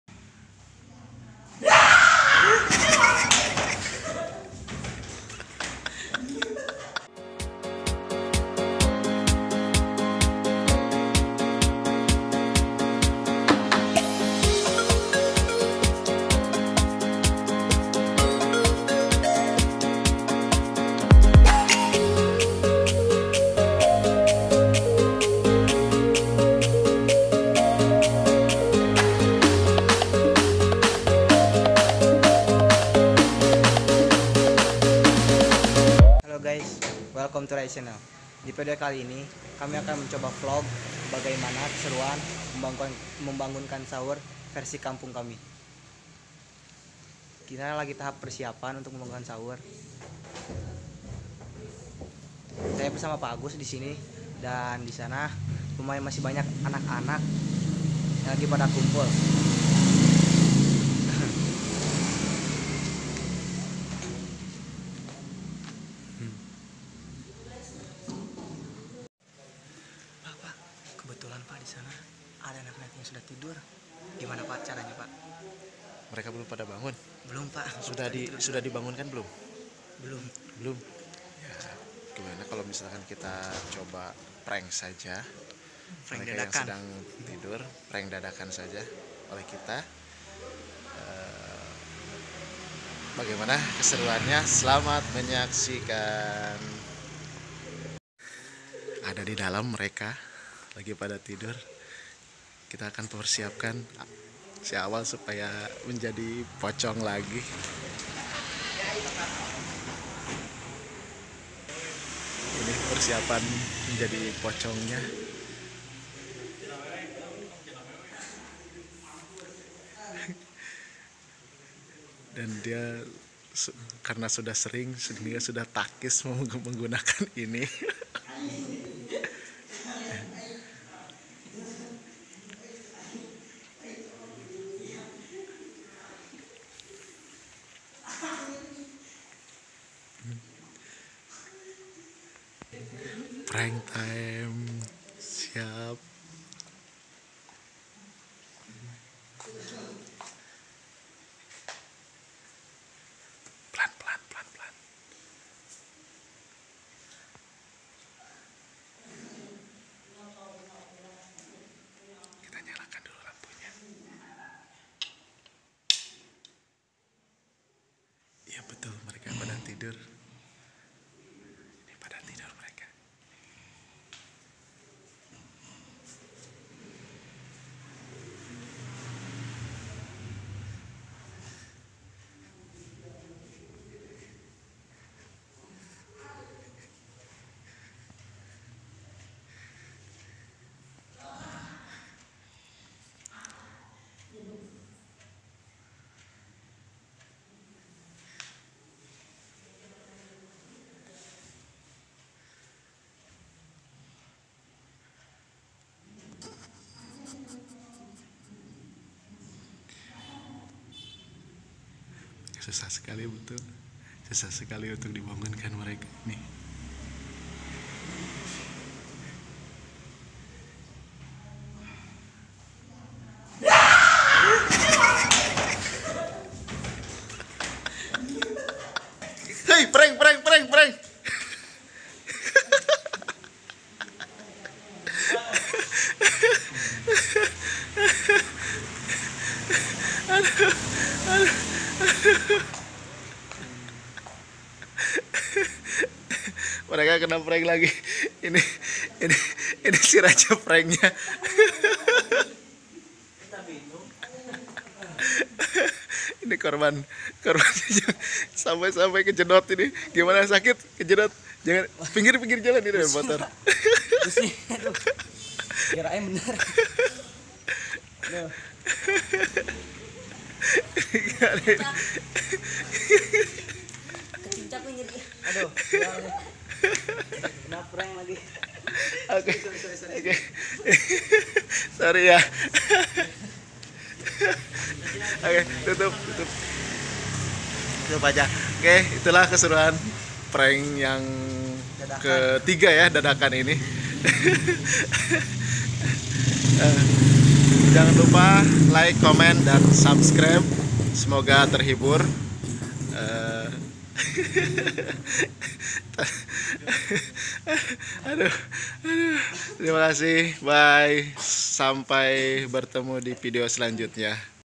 PRANK!!! POCONG sampai benjol kejedot tiang..😂🤣🤣🤣
Tadinya mau buat vidio keseruan membangunkan sahur..malah jd PRANK!! Mungpung ada kesempatan..selamat menyaksikan..😘